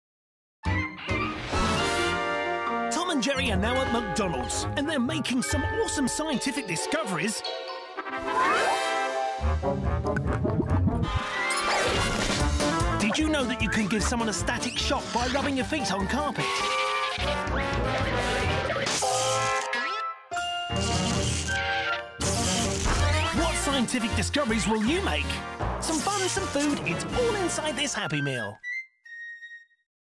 McDonald's Tom & Jerry Advert 2015
Here is an advert from McDonald's in the UK promoting the Tom & Jerry Happy Meal!